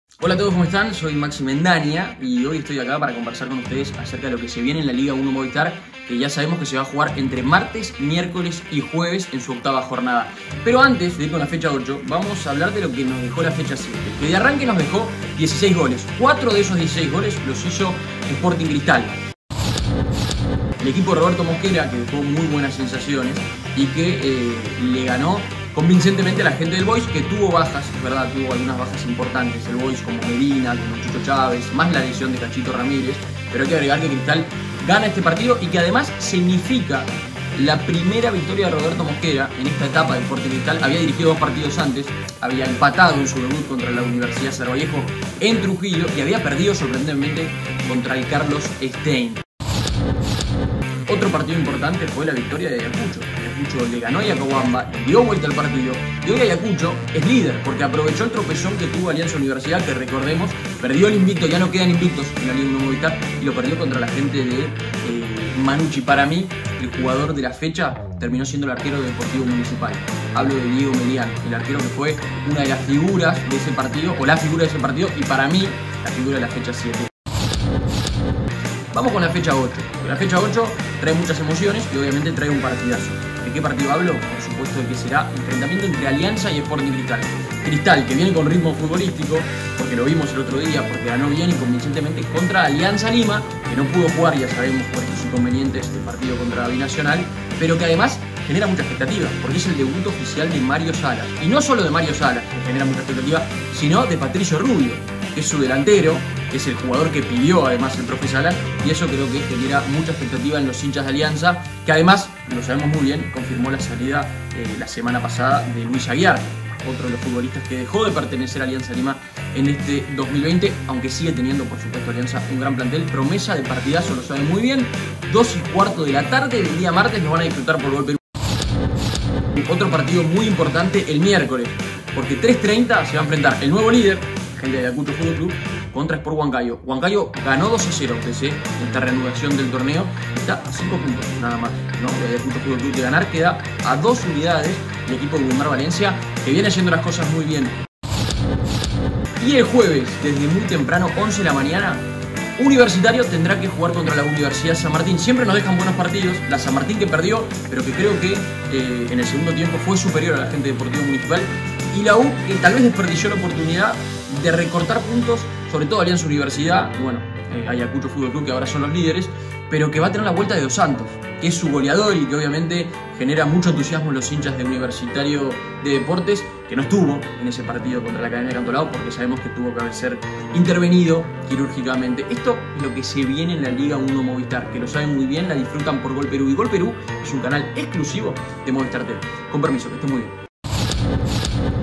Lo que se viene en la Liga1 Movistar
Maxi Mendaña da un repaso de la jornada 7 y nos cuenta qué es lo que se viene en la jornada 8 de la Liga1 Movistar.